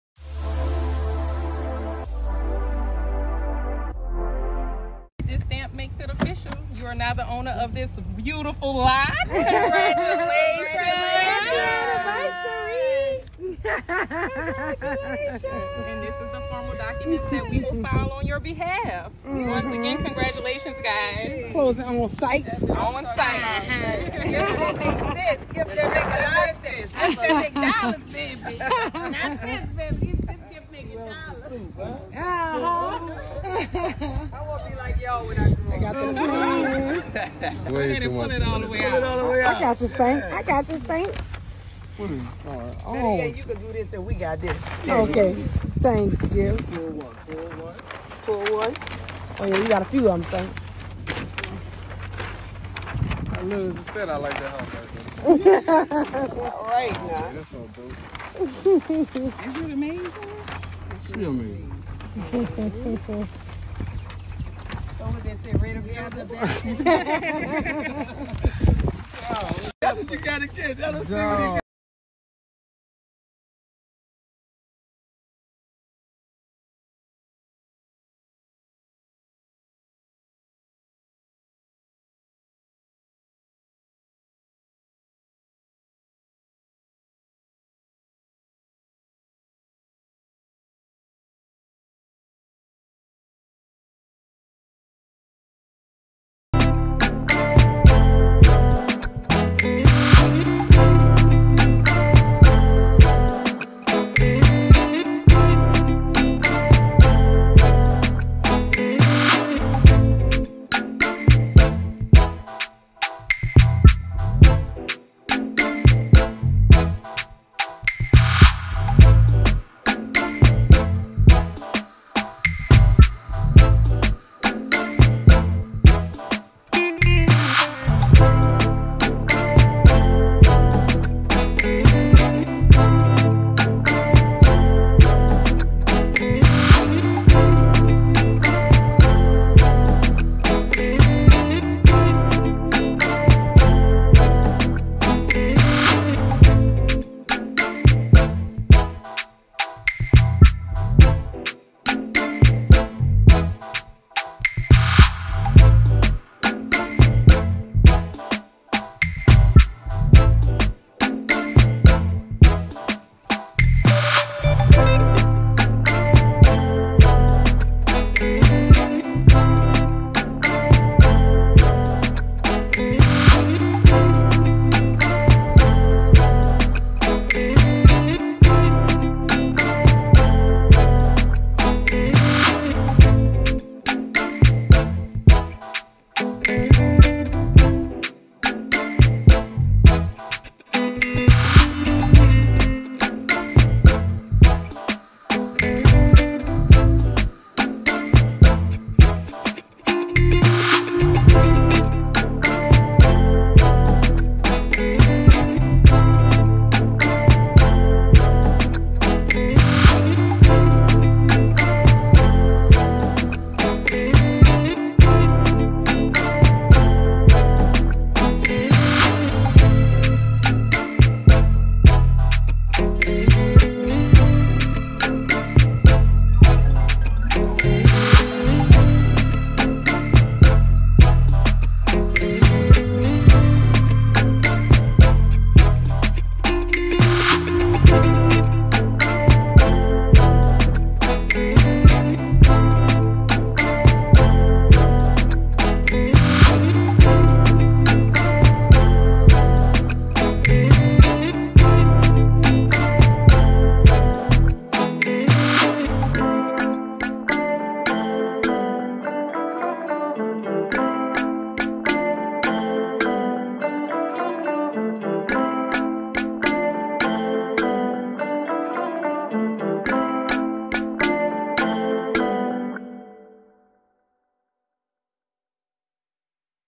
Supa Cent buys land for her boyfriend, he proposes
Supa Cent, the Crayon Case creator,  buys land for her boyfriend, he returns the gesture and proposes.